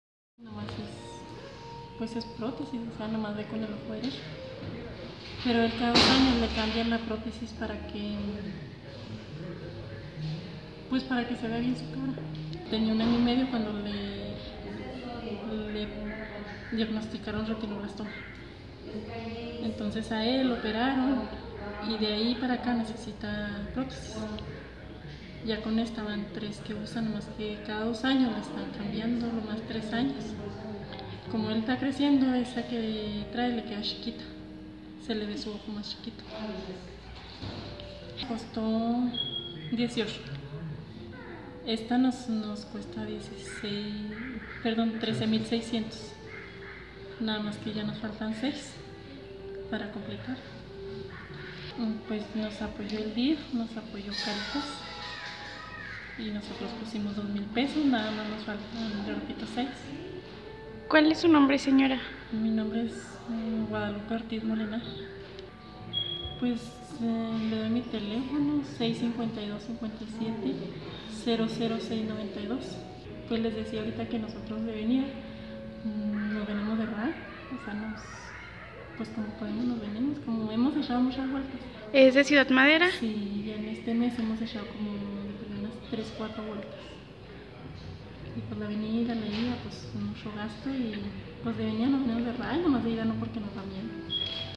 Solicitan apoyo para comprar prótesis del niño Gabriel Leon
La familia León Ortiz, hace un llamado a la ciudadanía, con la finalidad de solicitar su apoyo y recaudar fondos para poder adquirir una prótesis óptica, así como cubrir los gastos de la operación de su hijo Gabriel León Ortiz, quien desde que cumplió un año de edad fue diagnosticado con Retinoblastoma.